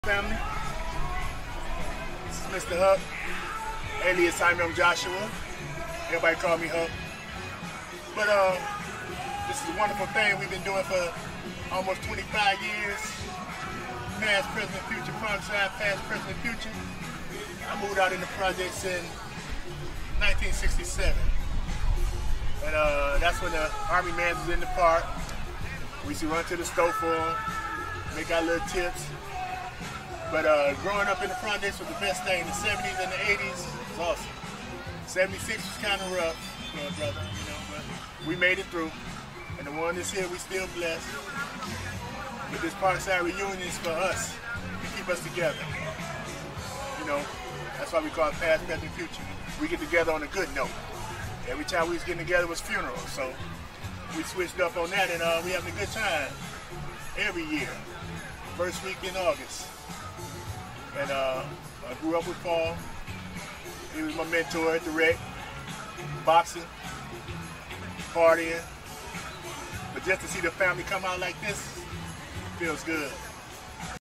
Parkside Historical Society Presents Parkside 24th Annual Reunion: View Description Box For Weblink
Parkside Historical Society Presents Parkside 24th Annual Reunion: View Comments/Description Box For Weblink

Select Menu in the top screen corner, scroll down to the “LEGACY” tab link (To view your video selections)

(Video/Website Archives Credit Paul R. Robinson)

Parkside Historical Society Presents…
Parkside 24th Annual "2023” PARKSIDE Community, Family & Friends Picnic Reunion… At Chandler Park, Michigan